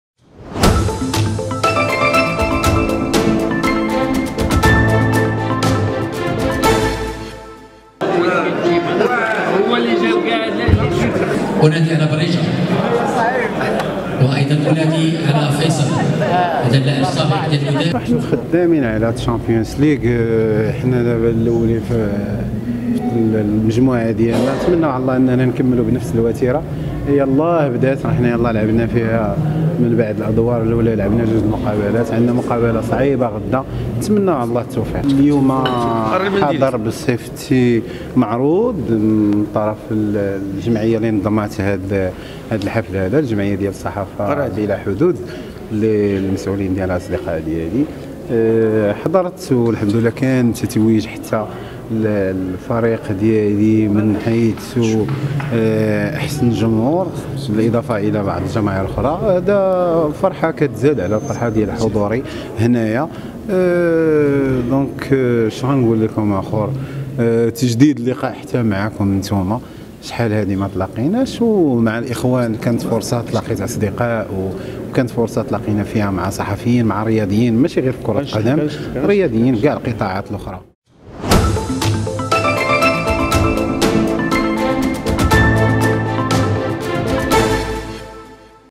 رئيس فريق الرجاء الرياضي: هدفنا هذا الموسم الفوز بعصبة الأبطال الإفريقية
أكد عزيز البدراوي، رئيس فريق الرجاء الرياضي أن فريقه سينافس هذا الموسم على كأس عصبة الأبطال الإفريقية